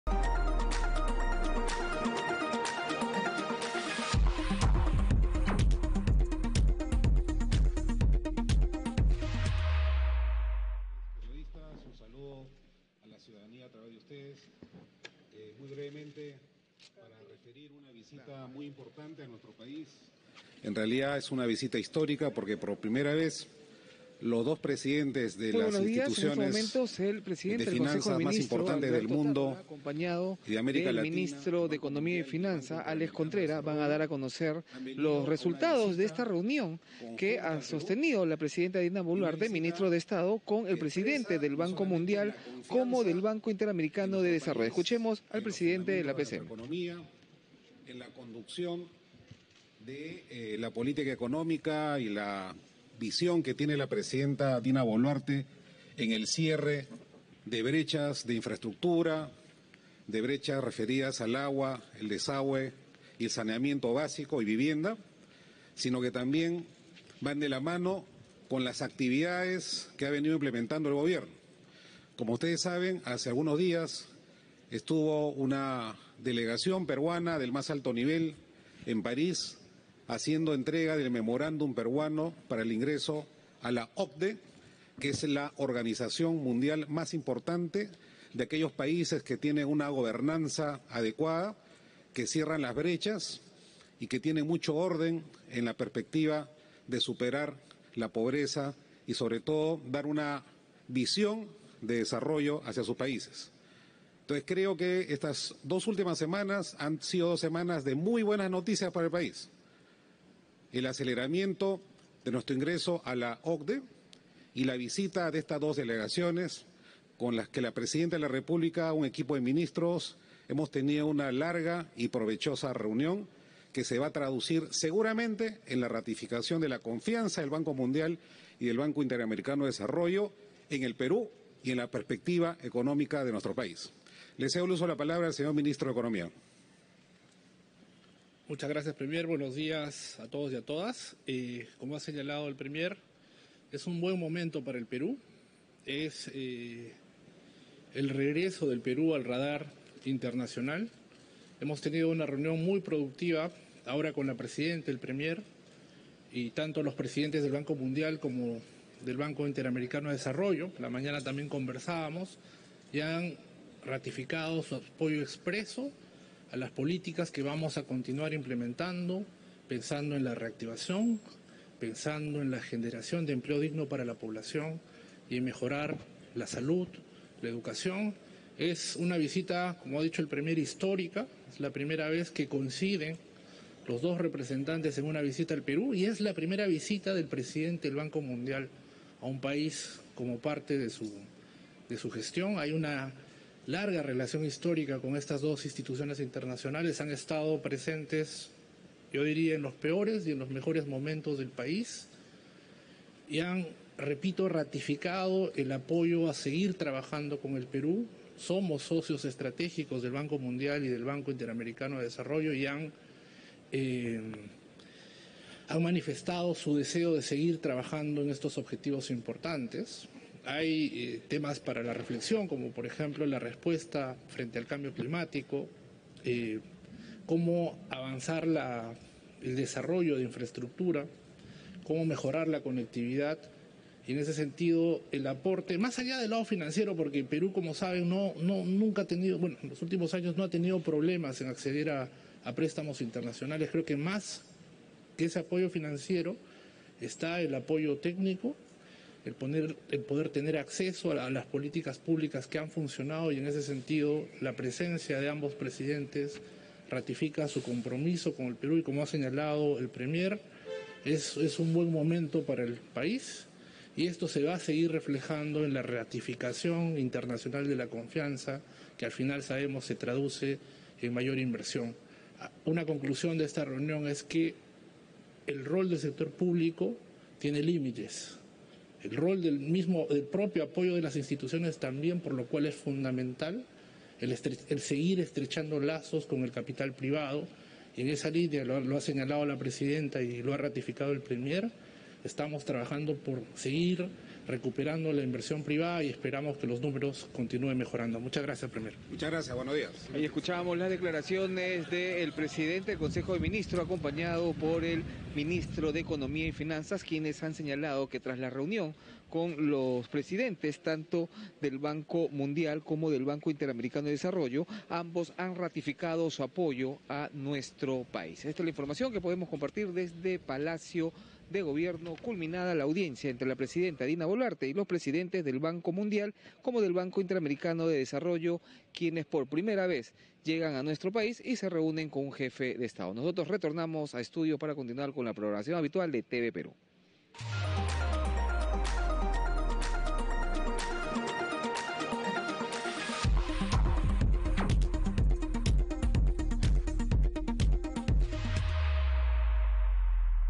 Cobertura Especial | Dina Boluarte se reunió con los titulares del BM y del BID
Ministros brindan conferencia de prensa tras reunión de jefa de Estado, Dina Boluarte, con el presidente del Banco Mundial, Ajay Banga, y el presidente del Banco Interamericano de Desarrollo, Ilan Goldfajn.